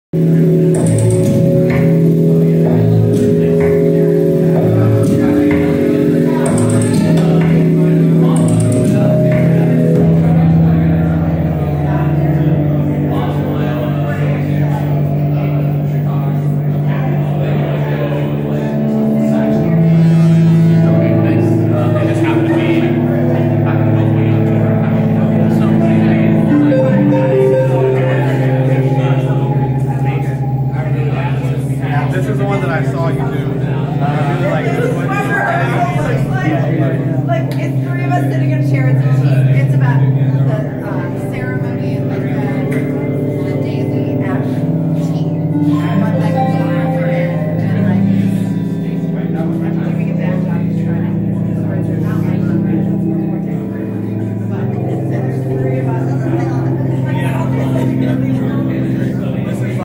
lonesav: LONE RADIO 1, 2022 @FEEDart (opening reception)
peep the opening reception for Alex "lonesav" Staley's exhibition "LONE RADIO 1" in FEED 0NE Sandbox project space.